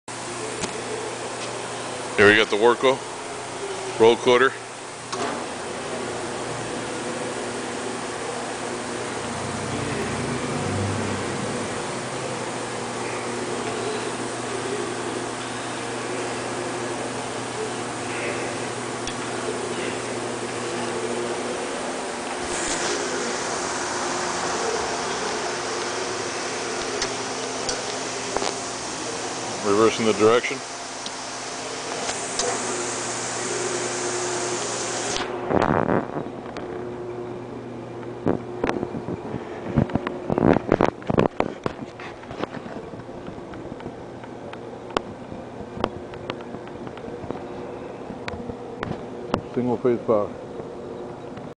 Warco Roll Coater
Running this Walco Roll Coater. Runs great just needs a little TLC which is our specialty.
SINGLE TOP COATER
CAPACITY 52” WIDE
5” DIAMETER ROLLS